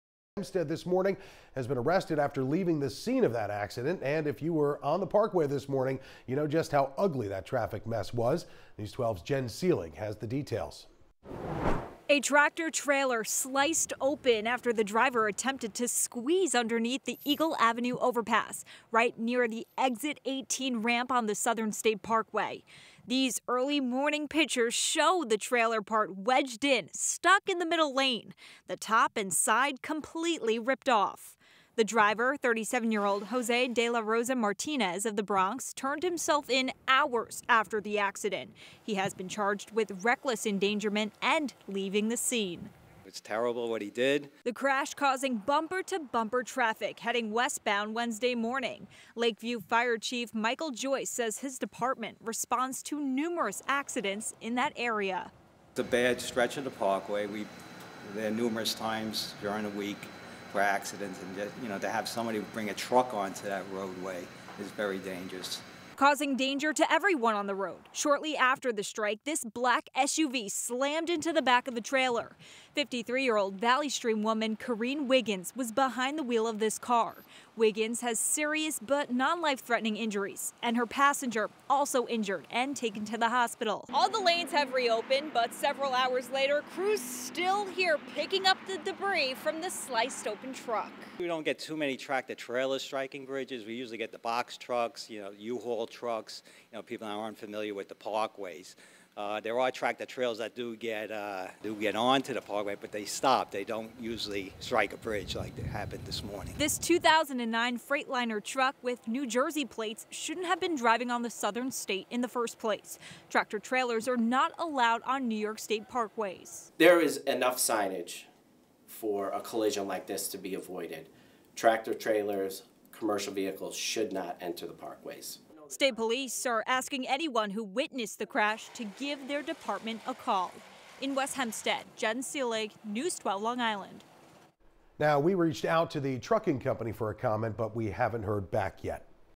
Driver of tractor-trailer who struck overpass on Southern State arrested after leaving scene
According to police, the incident happened around 3:48 a.m. in Lakeview.